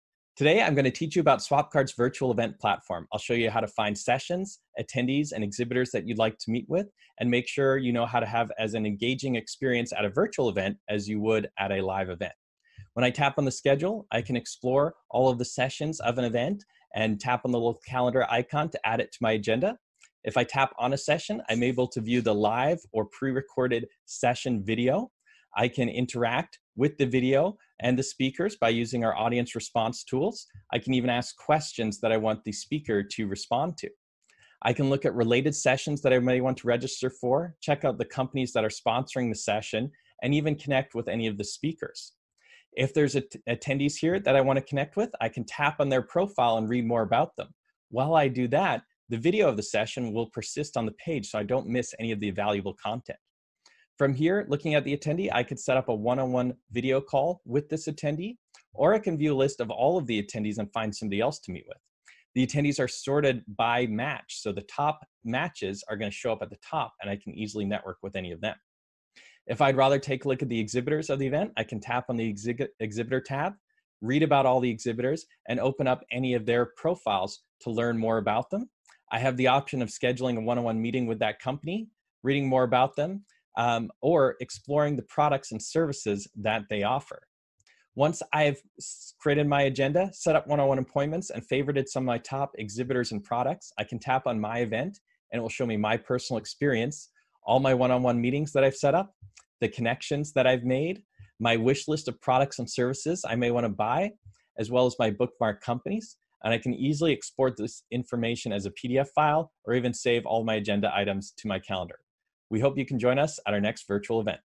Swapcard explicado en 90 segundos
Ingresá a Swapcard a través de para disfrutar todas las charlas y workshops de nerdearla.

En el evento vamos a utilizar Swapcard como plataforma principal y te dejamos acá un breve video para que conozcas a fondo cómo podés utilizarla!

Cualquier pregunta que tengas al respecto, escribinos a produccion arroba sysarmy punto com.